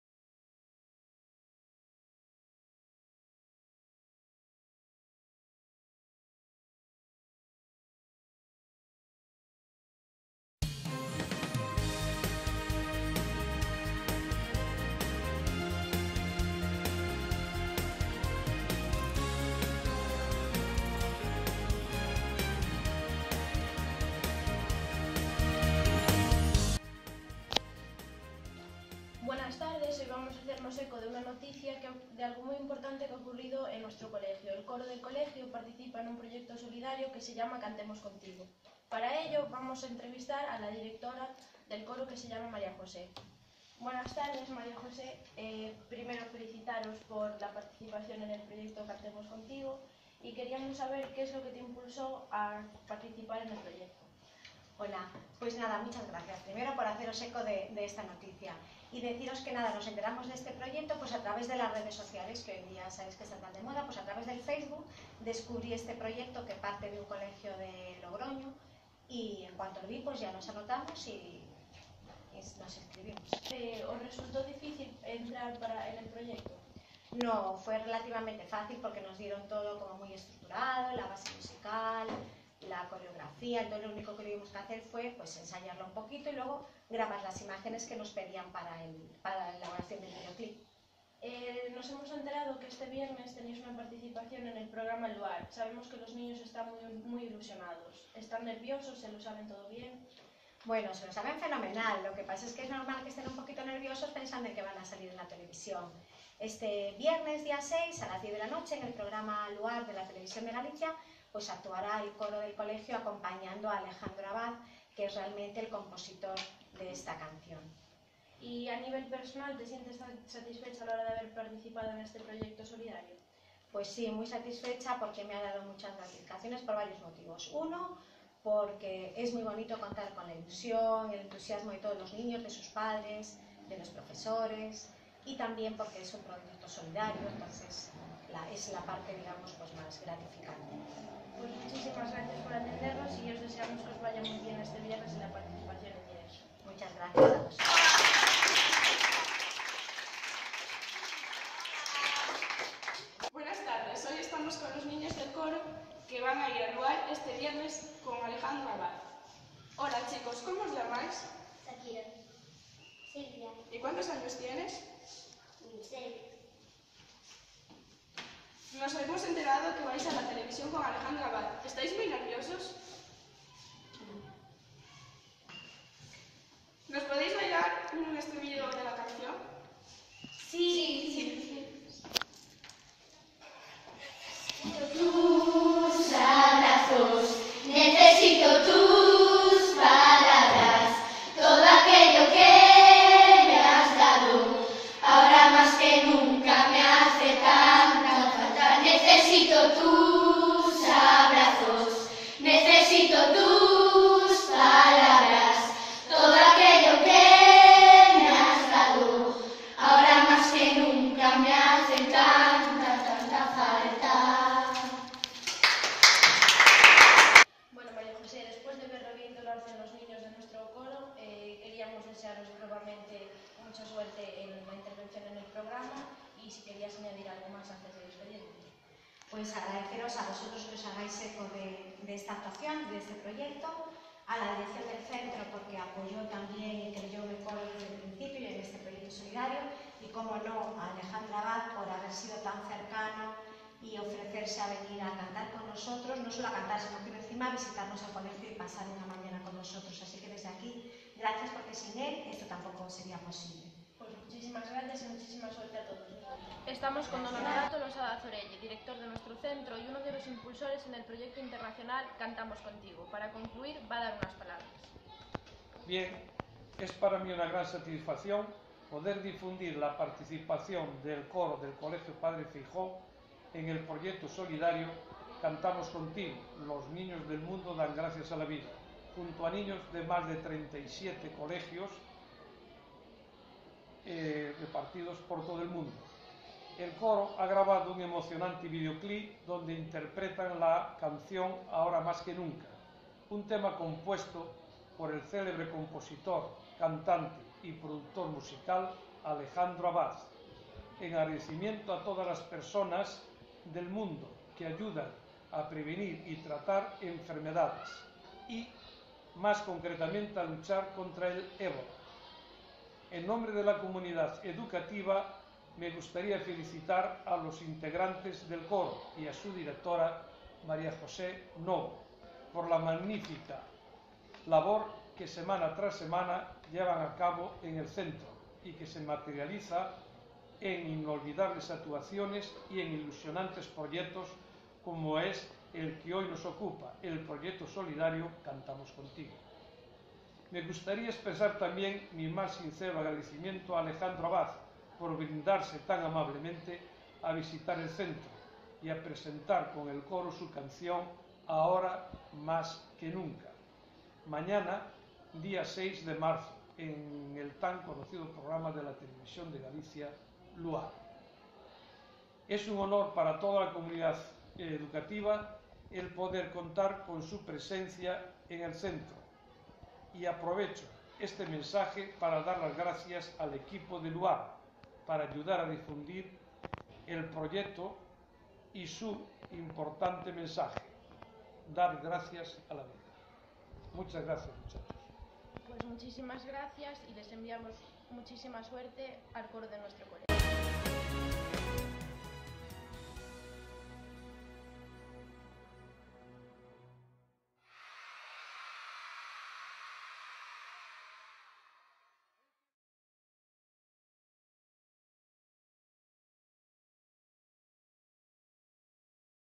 Alejandro Abad con el Colegio Padre Feijoo Zorelle - Promoción
Anuncio de la visita de Alejandro Abad al colegio Padre Feijoo Zorelle y de su actuación en la TVG el 6 de Marzo de 2015 junto al coro del Colegio.